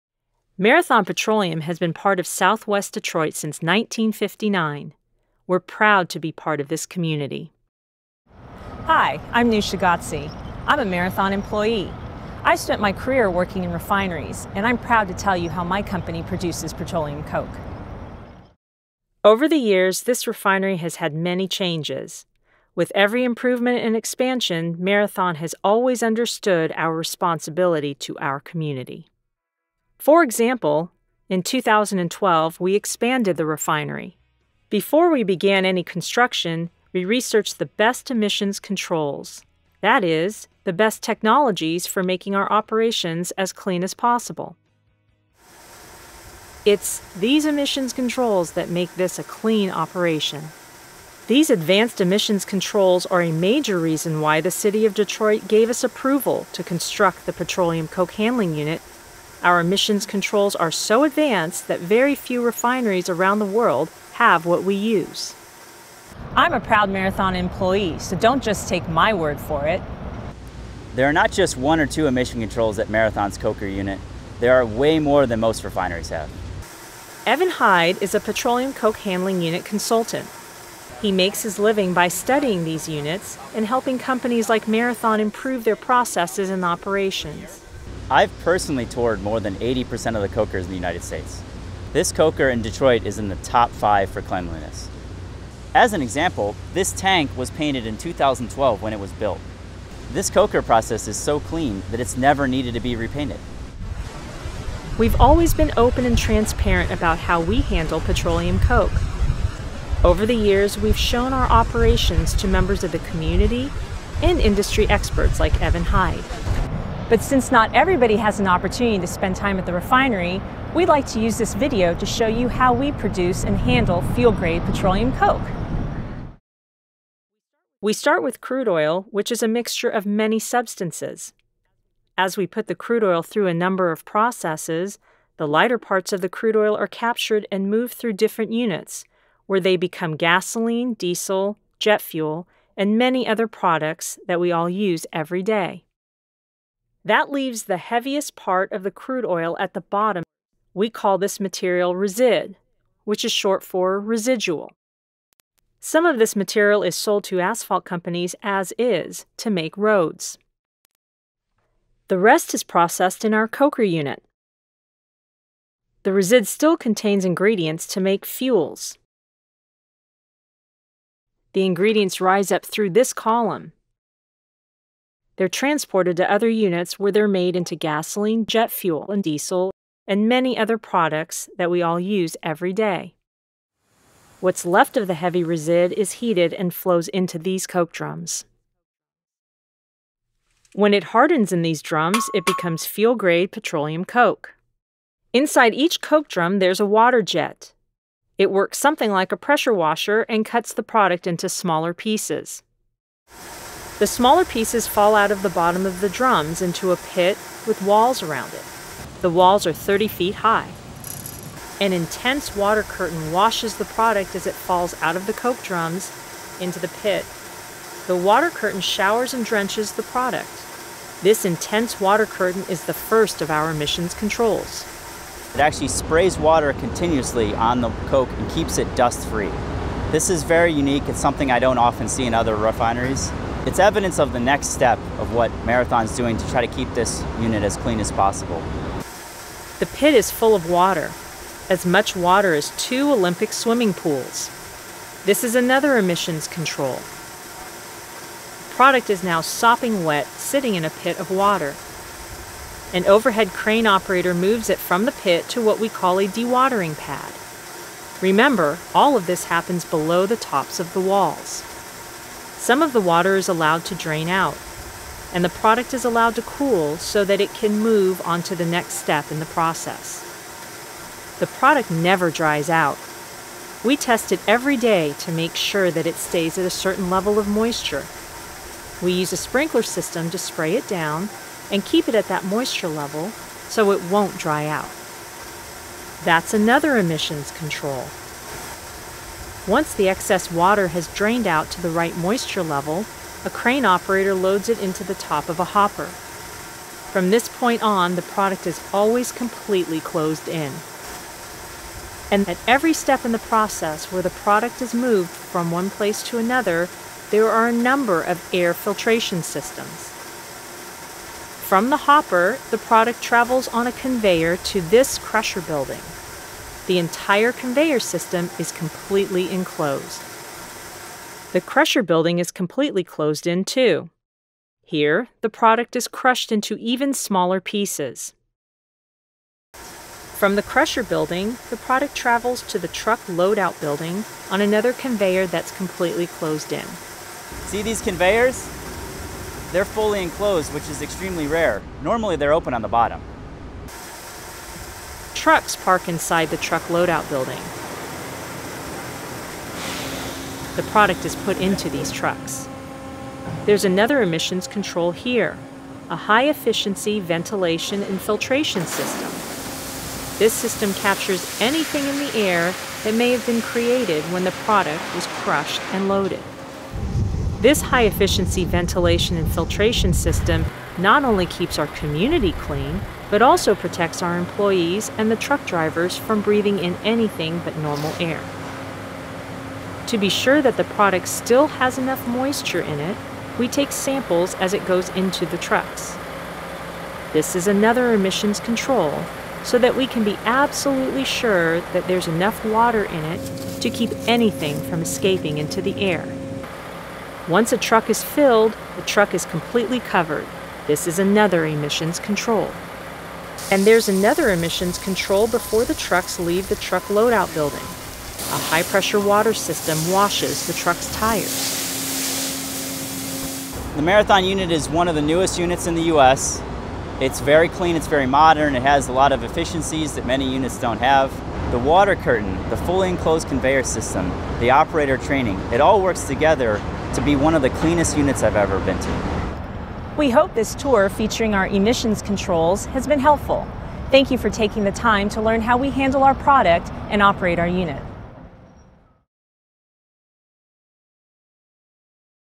Detroit refinery's petroleum coke processing unit (English)
Marathon Petroleum’s Detroit refinery is home to a petroleum coke processing unit that allows for safe, efficient and clean use during the refining process. Learn more about this state-of-the-art facility, and how we eliminate fugitive dust, here.